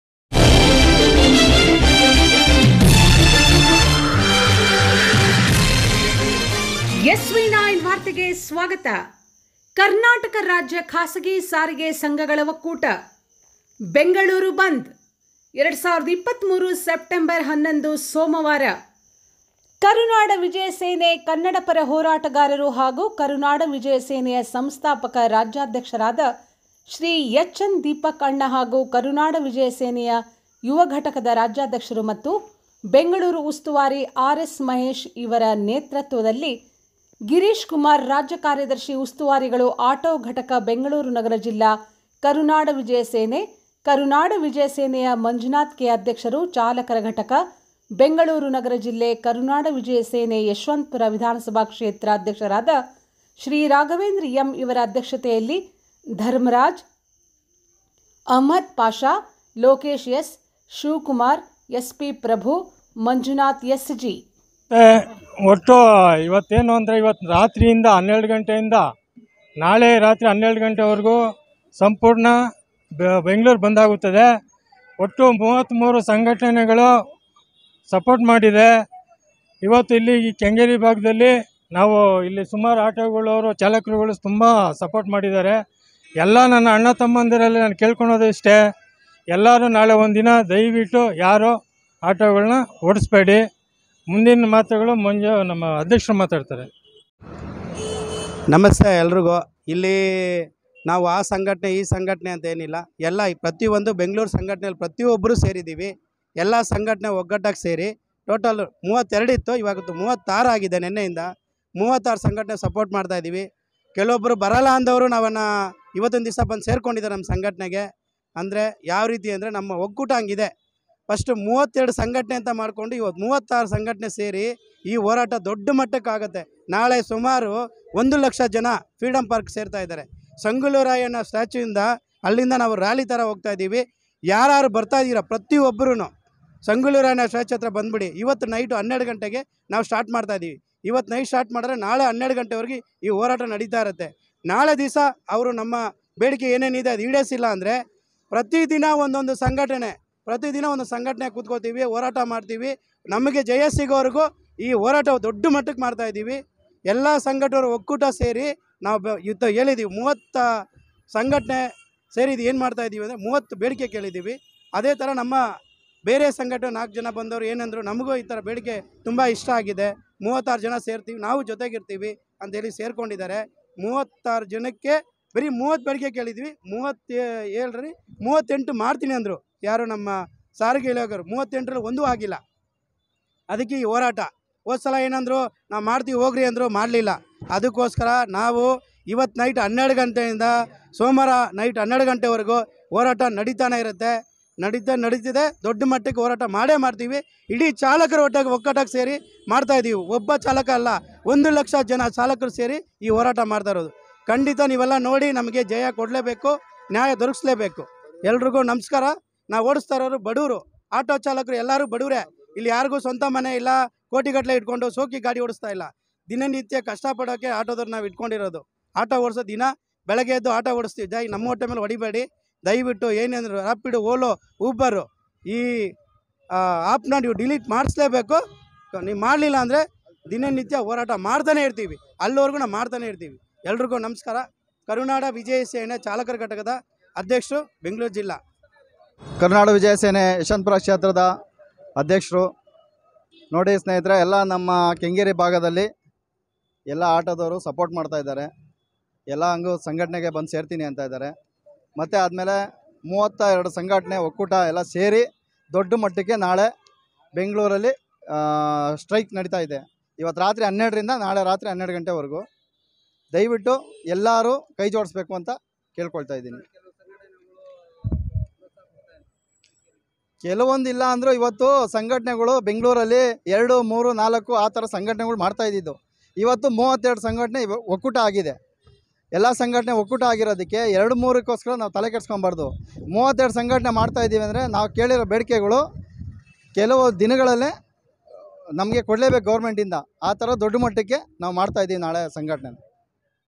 ಕರ್ನಾಟಕ ರಾಜ್ಯ ಖಾಸಗಿ ಸಾರಿಗೆ ಸಂಘಗಳ ಒಕ್ಕೂಟಬೆಂಗಳೂರು ಬಂದ್ ಕರುನಾಡ ವಿಜಯಸೇನೆ ಕನ್ನಡಪರ ಹೋರಾಟಗಾರರು ಹಾಗೂ ಕರುನಾಡ
ಕರ್ನಾಟಕ ರಾಜ್ಯ ಖಾಸಗಿ ಸಾರಿಗೆ ಸಂಘಗಳ ಒಕ್ಕೂಟ
ಬೆಂಗಳೂರು ಬಂದ್ ಕರುನಾಡ ವಿಜಯಸೇನೆ ಕನ್ನಡಪರ ಹೋರಾಟಗಾರರು ಹಾಗೂ ಕರುನಾಡ ವಿಜಯಸೇನೆಯಕರುನಾಡ ವಿಜಯಸೇನೆ ಕನ್ನಡಪರ ಹೋರಾಟಗಾರರು ಹಾಗೂ ಕರುನಾಡ ವಿಜಯಸೇನೆಯ ಸಂಸ್ಥಾಪಕ ರಾಜ್ಯಾಧ್ಯಕ್ಷರಾದ ಶ್ರೀ ಹೆಚ್. ಎನ್. ದೀಪಕ್ ಅಣ್ಣ ಹಾಗೂ ಕರುನಾಡ ವಿಜಯ ಸೇನೆಯ ಯುವ ಘಟಕದ ರಾಜ್ಯಾಧ್ಯಕ್ಷರು ಮತ್ತು ಬೆಂಗಳೂರು ಉಸ್ತುವಾರಿ ಆರ್. ಎಸ್ .ಮಹೇಶ್ ಇವರ ನೇತೃತ್ವದಲ್ಲಿ ಗಿರೀಶ್ ಕುಮಾರ್  ರಾಜ್ಯ ಕಾರ್ಯದರ್ಶಿ ಉಸ್ತುವಾರಿಗಳು ಆಟೋ ಘಟಕ ಬೆಂಗಳೂರು ನಗರ ಜಿಲ್ಲಾ ಕರುನಾಡ ವಿಜಯ ಸೇನೆ, ಕರುನಾಡ ವಿಜಯ ಸೇನೆಯ ಮಂಜುನಾಥ್ ಕೆ. ಅಧ್ಯಕ್ಷರು ಚಾಲಕರ ಘಟಕ, ಬೆಂಗಳೂರು ನಗರ ಜಿಲ್ಲೆ ಕರುನಾಡ ವಿಜಯ ಸೇನೆ ಯಶವಂತಪುರ ವಿಧಾನಸಭಾ ಕ್ಷೇತ್ರ ಅಧ್ಯಕ್ಷರಾದ ಶ್ರೀ ರಾಘವೇಂದ್ರ ಎಂ ಇವರ ಅಧ್ಯಕ್ಷತೆಯಲ್ಲಿ    ಧರ್ಮರಾಜ್
 ಅಮ್ಜದ್ ಪಾಷ
 ಲೋಕೇಶ್ ಎಸ್
 ಶಿವಕುಮಾರ್
 ಎಸ್ ಪಿ ಪ್ರಭು
 ಮಂಜುನಾಥ್ ಎಸ್ ಜಿ